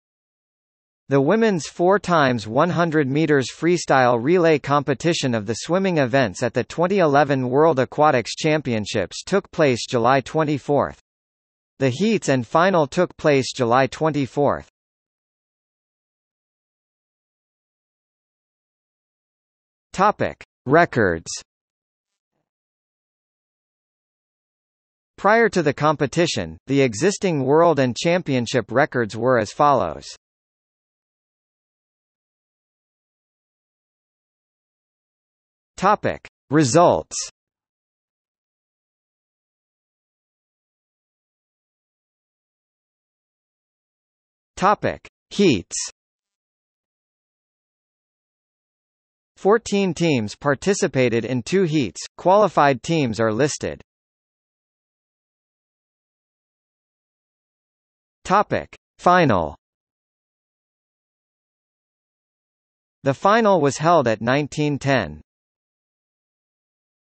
Swimming at the 2011 World Aquatics Championships – Women's 4 × 100 metre freestyle relay | W ...
00:00:16 1 Records
00:00:32 2 Results
00:00:41 2.1 Heats
00:00:54 2.2 Final

- Socrates

SUMMARY
The women's 4×100 metre freestyle relay competition of the swimming events at the 2011 World Aquatics Championships took place July 24. The heats and final took place July 24.